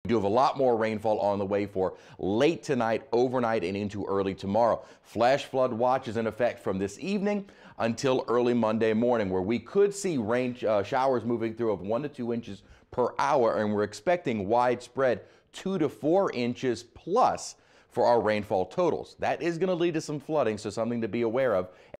Heavy rains, flooding possible Sunday night/Monday
Meteorologist Chris Franklin talks about the possibility of heavy rain and flooding in the metro area overnight.